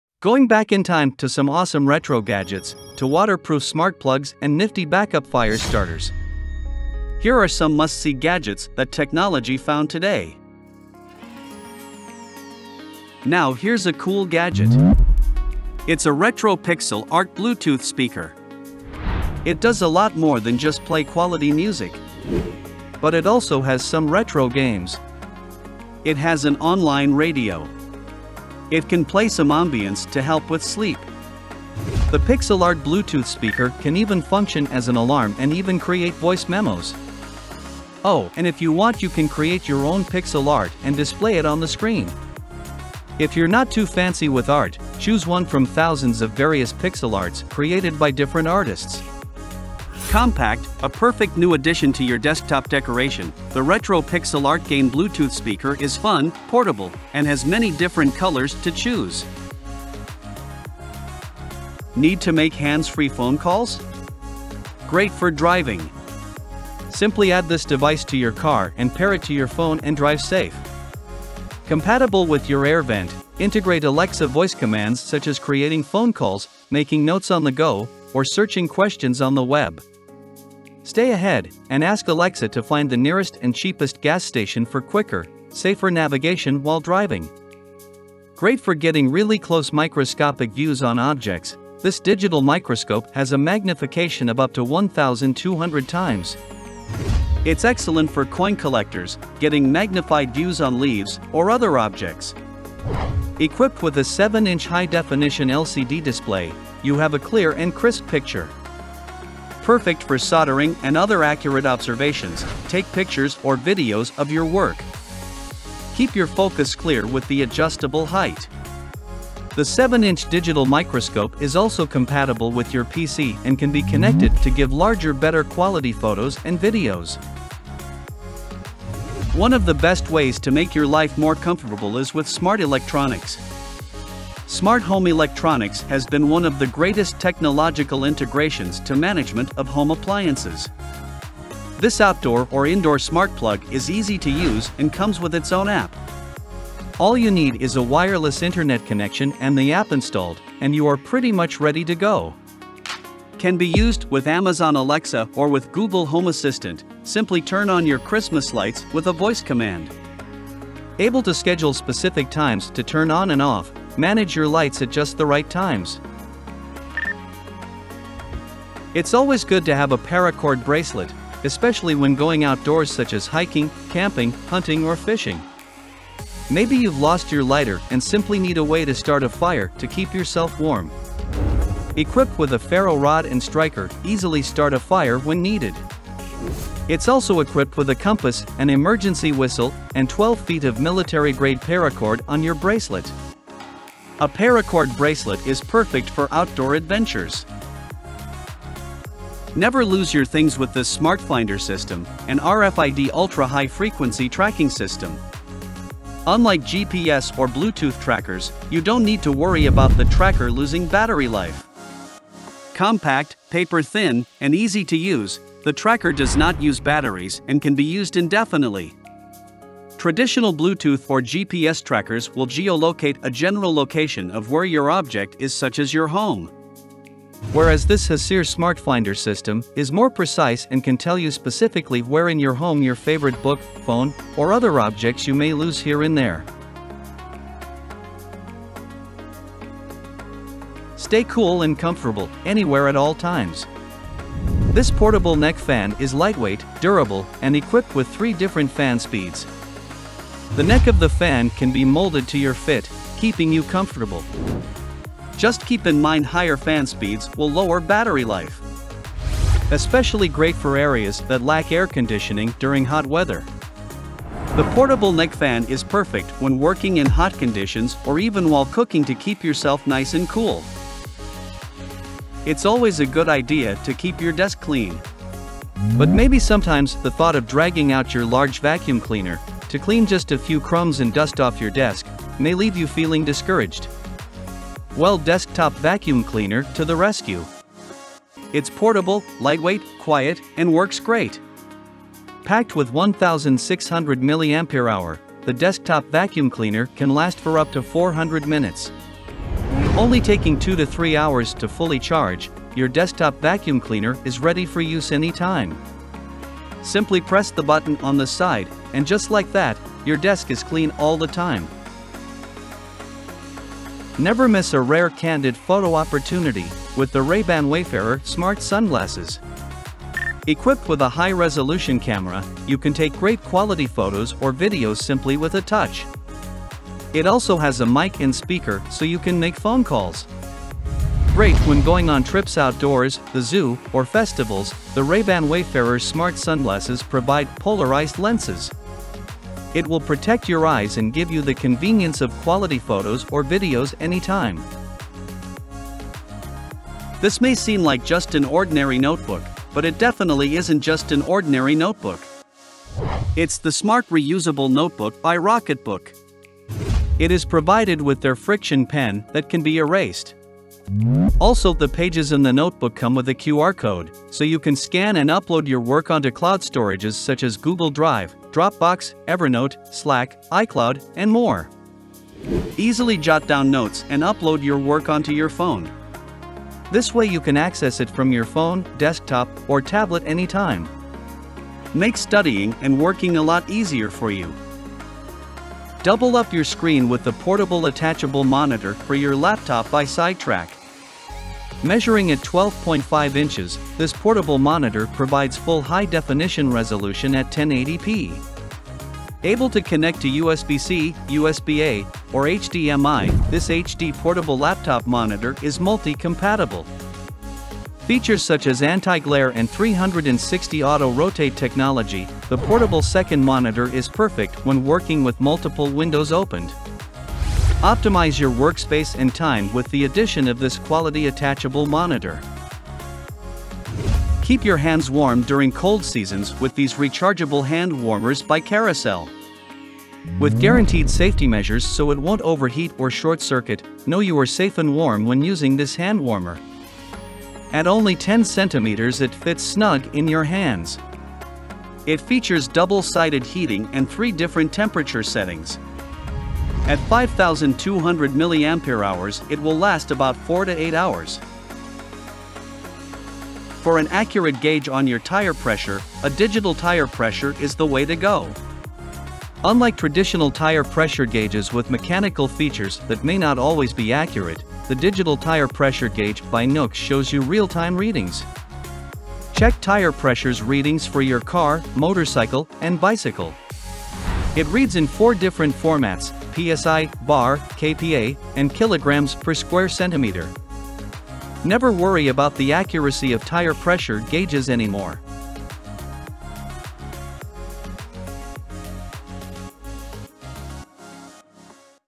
13 Awesome Gadgets from Amazon
Going back in time to some awesome retro gadgets, to waterproof smart plugs and nifty backup fire starters. Here are some must see gadgets that technology found today.

0:00 Introduction
0:13 | Divoom Retro Pixel Art Game Speaker
1:01 | Echo Auto Hands-free Alexa
1:28 | 7'' Digital Microscope
2:09 | Minoston Outdoor Smart Plug
2:48 | Atomic Bear Paracord Bracelet
3:19 | Hecere Smart Finder System
4:00 | Portable Neck Fan
4:44 | ODISTAR Desktop Vacuum cleaner
5:14 | Ray-Ban Stories Smart Sunglasses
5:47 | Rocketbook Smart Reusable Notebook
6:25 | SideTrak Attachable Monitor for Laptop
7:04 | Karecel Rechargeable Hand Warmers
7:33 | NOOX Digital Tire Pressure Gauge